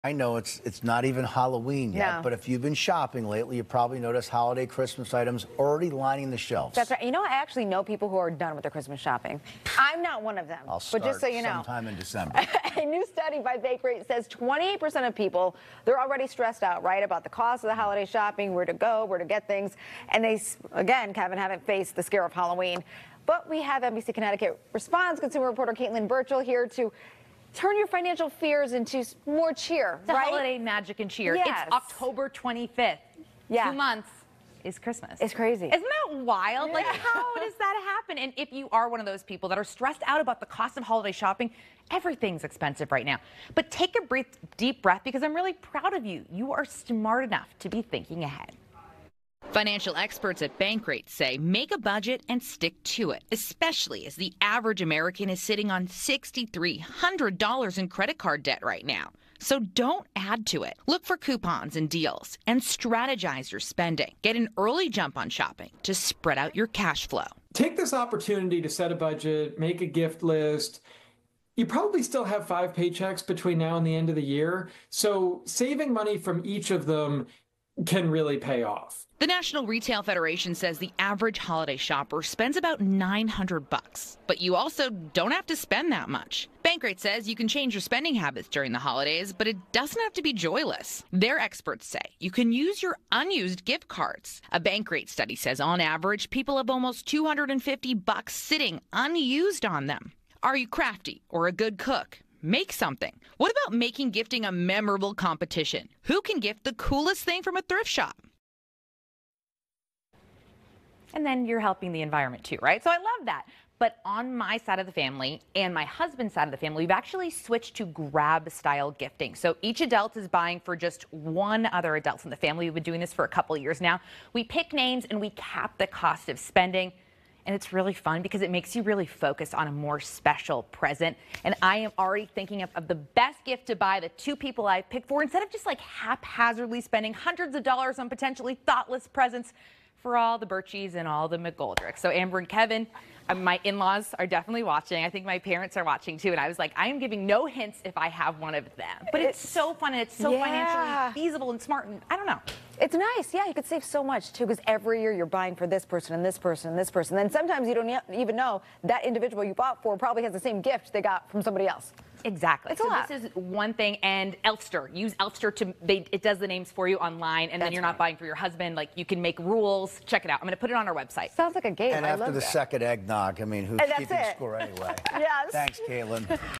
Here are some tips to avoid holiday shopping stress
A new study by Bankrate says 28% of people are already stressed out about the cost of holiday shopping. Here's how you can turn your financial fear into cheer.